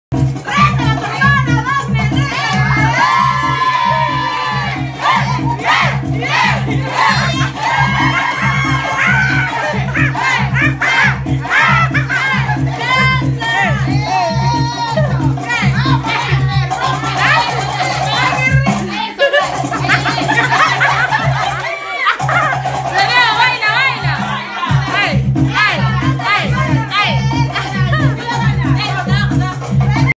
Cuando estas bailando y tu hermana te dejo chiquita 💪🎶
Peru-Callao   hermana menor deja mal ala mayor jejejeje